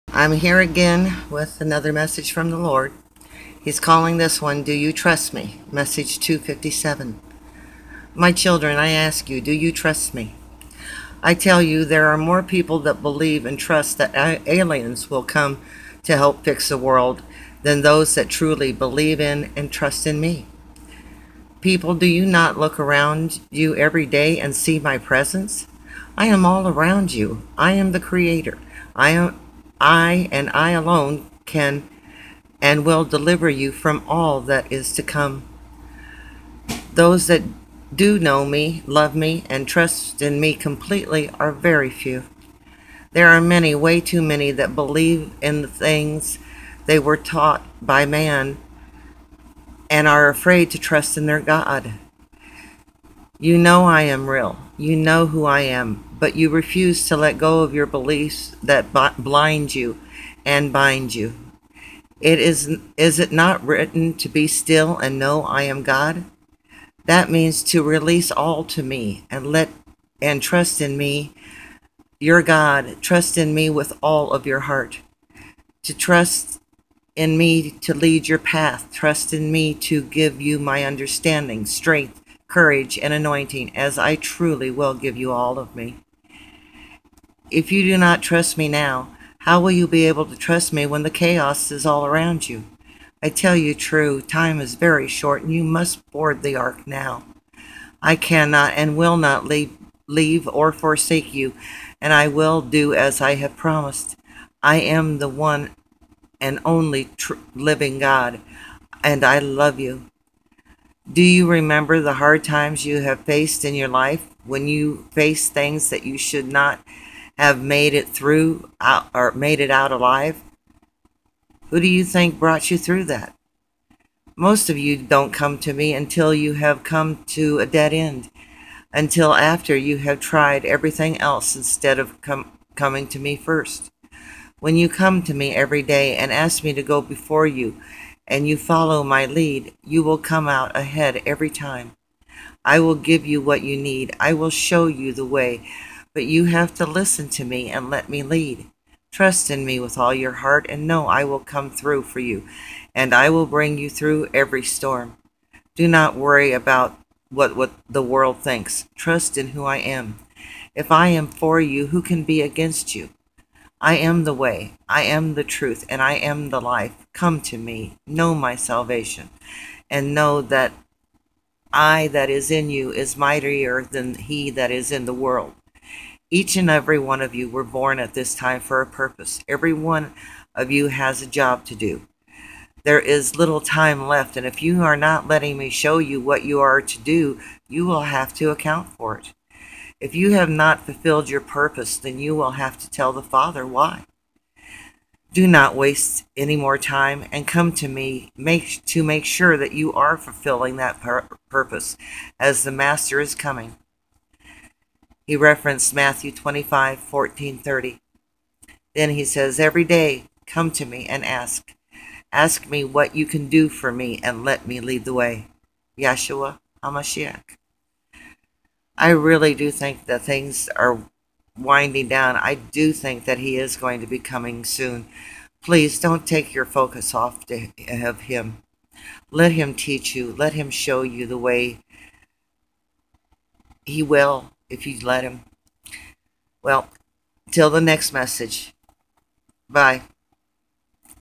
Do You Trust Me Message 257
My Children, I ask you, do you trust Me?  I tell you there are more people that believe, and trust that aliens will come to help fix the world than those that truly believe in, and trust in Me.
People do you not look around you every day and see My presence?  I Am all around you, I Am the creator, and I alone can, and will deliver you from all that is to come.  Those that do know Me, love Me, and trust in Me completely are very few.
There are many, way too many that believes in the things they were taught by man, and are afraid to trust in their God.  You know I am real, you know who I Am, but you refuse to let go of the beliefs that blind you, and bind you,.
Is it not written to be still and know I Am God, it means to release all to Me, and trust in Me your God, trust Me with all of your heart.
Trust in Me to lead your path, trust in Me to give you My understanding, strength, courage, and anointing as I truly will give you all of Me.
If you do not trust in Me now, how will you be able to trust in me when chaos is all around you?  I tell you true time is very short, and you must board the ark now.
I cannot, and will not leave or forsake you, and I will do as I have promised.  I Am the one and only living God, and I love you.
Do you remember the hard times you have faced in your life, when you faced things that you should not have made it out alive?  Who do you think brought you through?
Most of you don’t come to Me until you have come to a dead end, until after you have tried everything else instead of coming to Me first.
When you come to Me every day and ask Me to go before you, and you follow My lead, you will come out ahead every time.  I will give you what you need, I will show you the way, but you have to listen to Me, and let Me lead.
Trust in Me with all your heart and know I will come through for you, and I will bring you through every storm.  Do not worry about what the world thinks, trust in who I Am.
If I am for you who can be against you.  I am the way, I am the truth and I am the life, come to Me, know My salvation, and know, I that is in you is mightier than he that is in the world.
Each and every one of you were born at this time for a purpose, every one of you has a job to do.  There is little time left and if you are not letting Me show you what you are to do you will have to account for it.
If you have not fulfilled your purpose than you will have to tell the Father why.  Do not waste any more time and come to Me to make sure you are fulfilling that purpose as the Master Is Coming.   (Matt 25:14-30)
Every day come to Me and ask Me what you can do for Me and let Me lead the way.
Yeshua Hamashiach